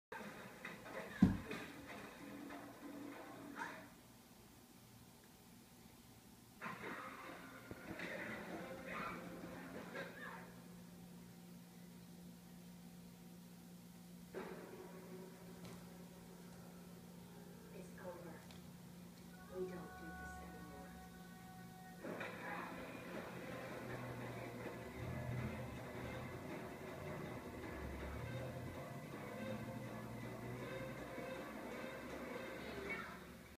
NewBox
Loki jumps into a new box to explore.